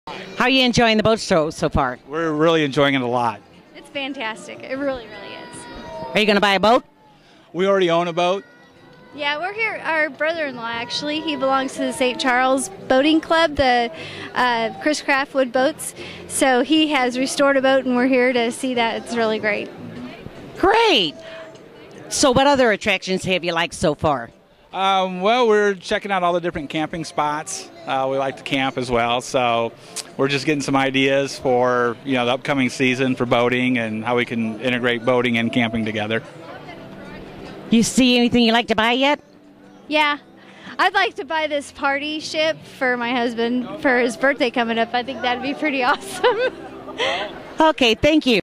Interview of a couple by the Pirate Ship
This was taken just after we got to the show by the Gypsy Rose Pirate Ship.  The couple had just gotten off the tour of the landship when Maureen talked to them.  We later ran into them at the Antique & Classic Boat Society display.  Very nice couple that were very patient with us as we were just getting setup.